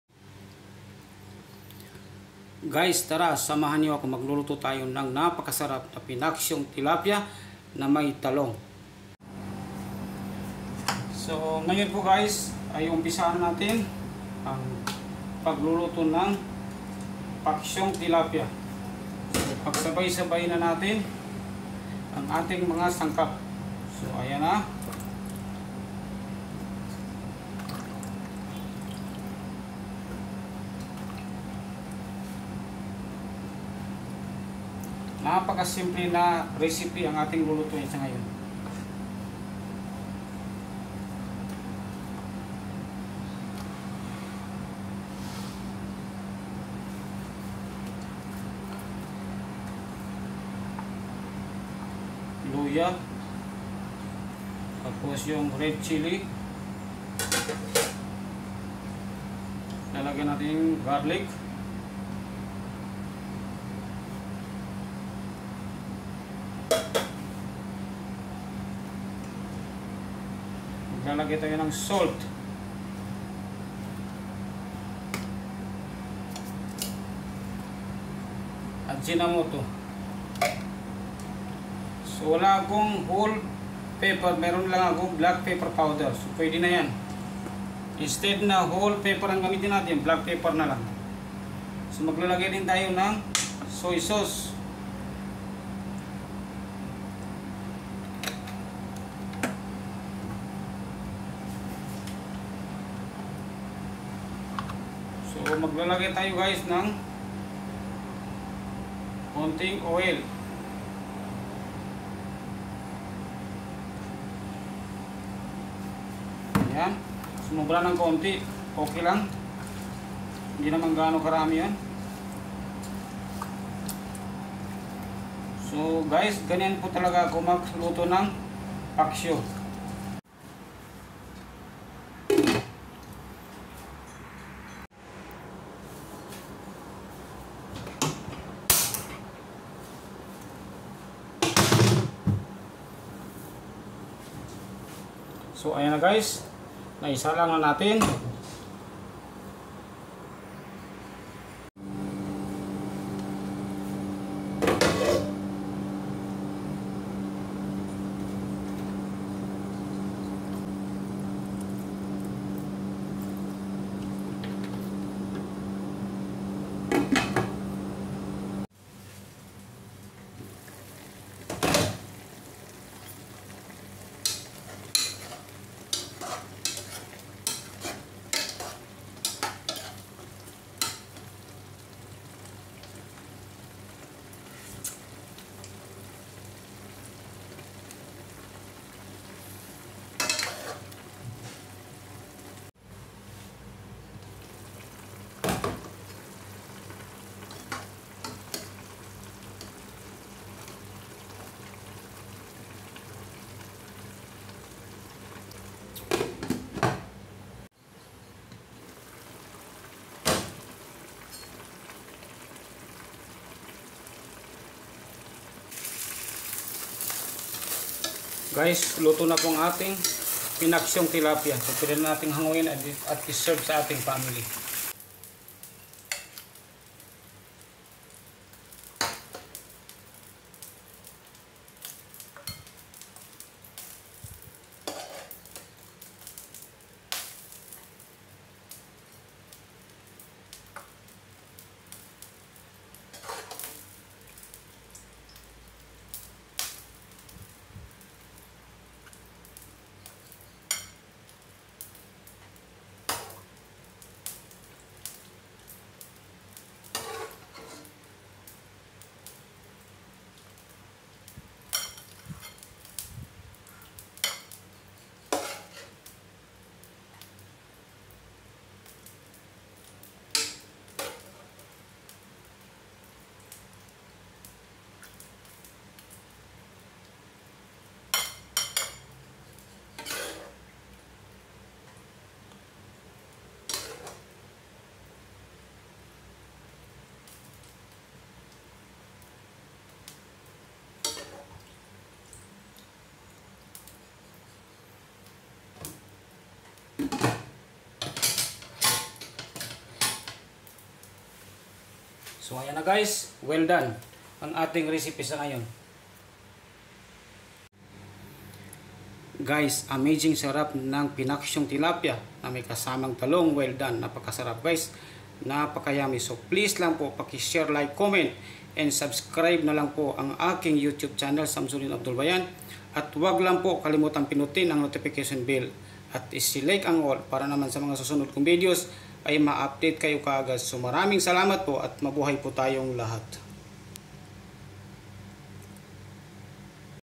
Paksiw Tilapia na may talong
mga sangkap
tilapia, talong, onion, garlic, ginger, red chilli, soy sauce, venigar,salt, msg, black pepper powder.cooking oil.